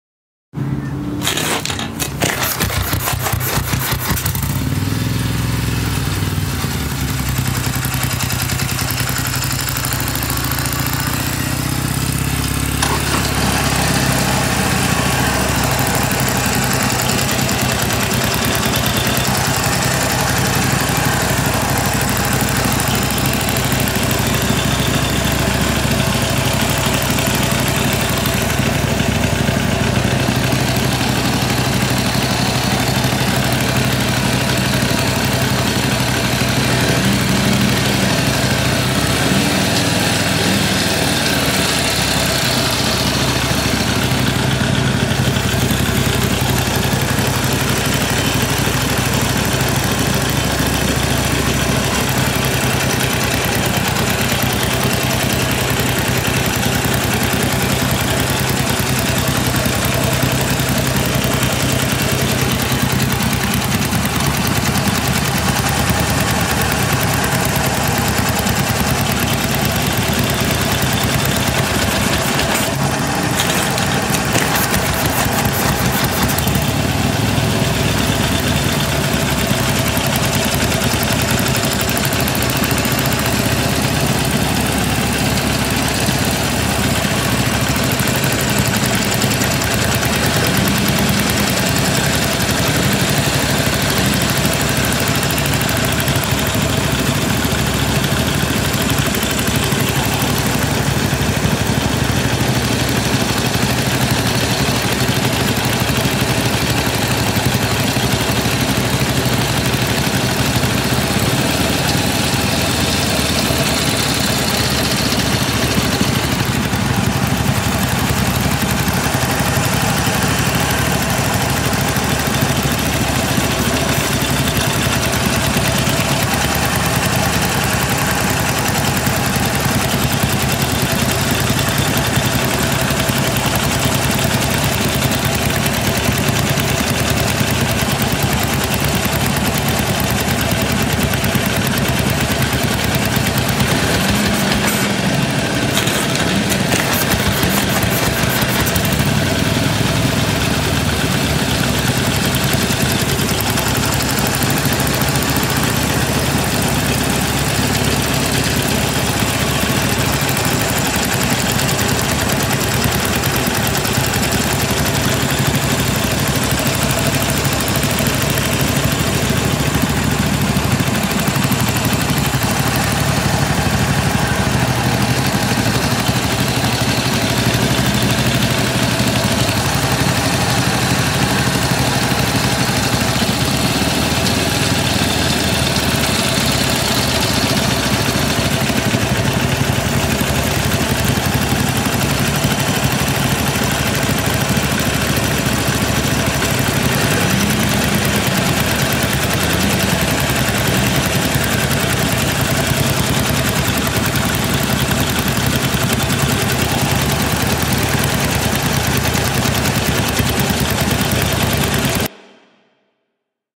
TABLEAU MOTEUR #2 OLIVIER MOSSET
Projection of a painting framed by the sound of a motorcycle engine.
Projection d'une peinture encadrée par le son d'un moteur de moto.
Links
Olivier Mosset Portrait
Point to Point Studio